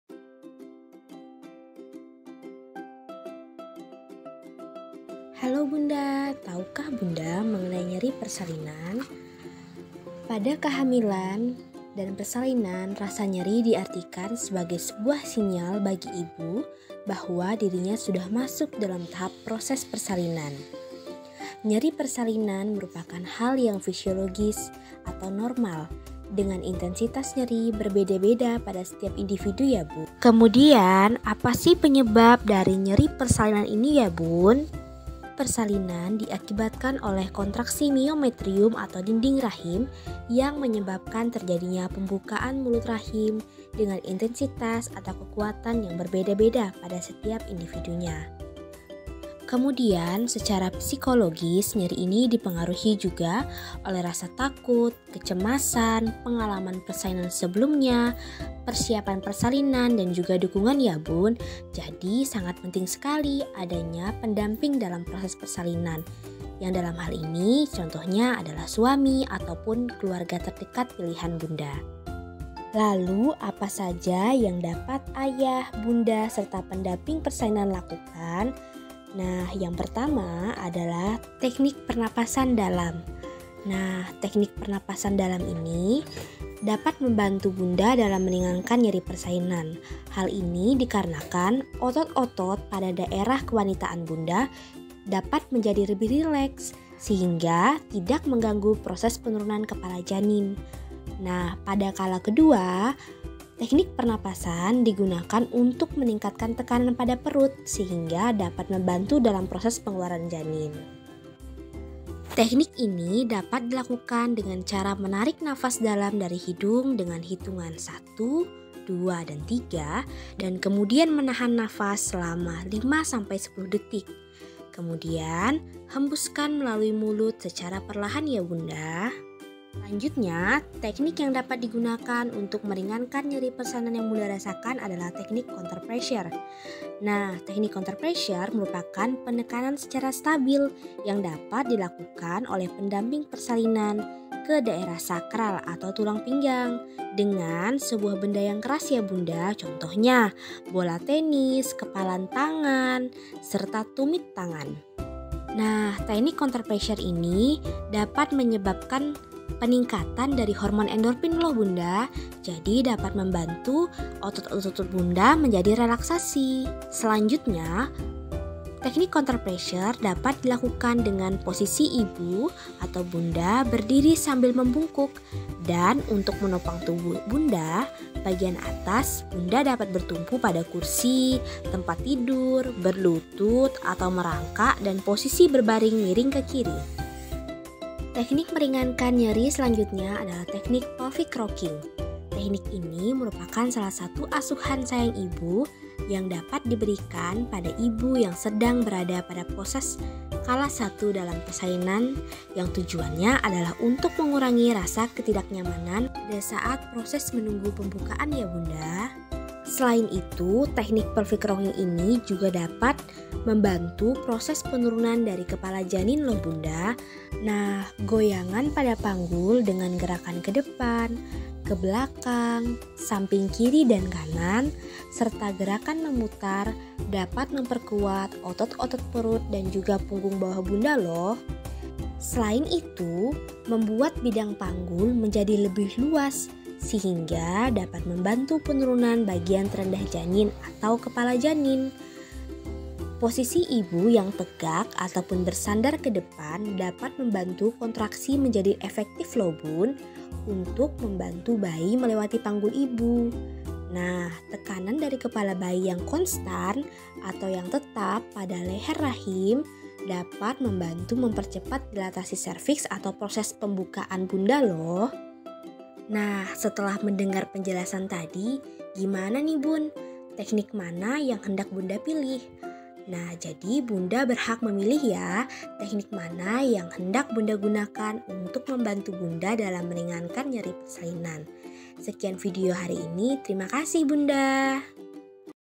Manajemen nyeri persalinan
Video manajemen nyeri persalinan merupakan video yang berisikan edukasi bagi ibu hamil dan juga ibu yang sedang dalam proses persalinan. Dalam video ini akan disajikan mengenai pengertian dari nyeri persalinan, penyebab serta solusi berupa macam- macam teknik yang dapat ibu , suami dan pendamping persalinan lakukan guna mengurangi nyeri dari persalinan yang sedang ibu rasakan.
Video ini disajikan dengan menggunakan audio penjelasan, musik berupa instrument serta beberapa gambar dan video ilustrasi yang diharapkan dapat sangat membantu ibu suami dan pendamping persalinan terkait pemahaman serta implementasi / praktik langsung.